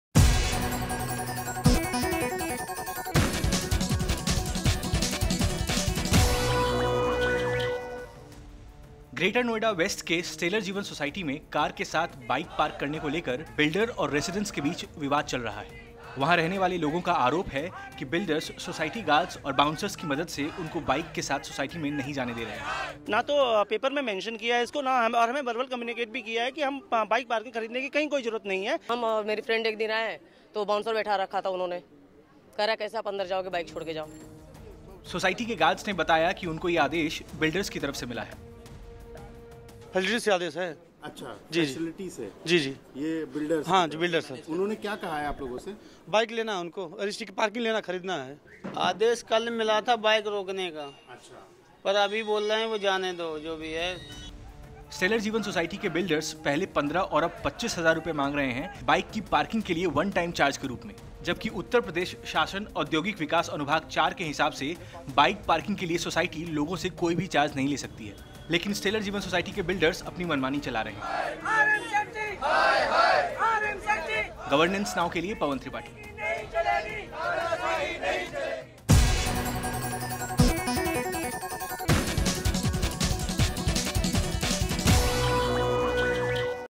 ग्रेटर नोएडा में कई बिल्डर्स कर रहे हैं मनमानी I Watch now Crime world Show
ग्रेटर नोएडा वेस्ट के स्टेलर जीवन सोसायटी में बाइक पार्किंग को लेकर बिल्डर और रेजिडेंट्स के बीच विवाद चल रहा है। बिल्डर्स बाइक पार्किंग के लिए वन टाइम चार्ज मांग रहे हैं जबकि नियमानुसार बाइक पार्किंग के लिए बिल्डर्स कोई चार्ज नहीं ले सकते। surag || Doordarshan || Number one || Detective || Suspense || Thrillers ||
Sudesh beri || || actor || murder mystery || # best serial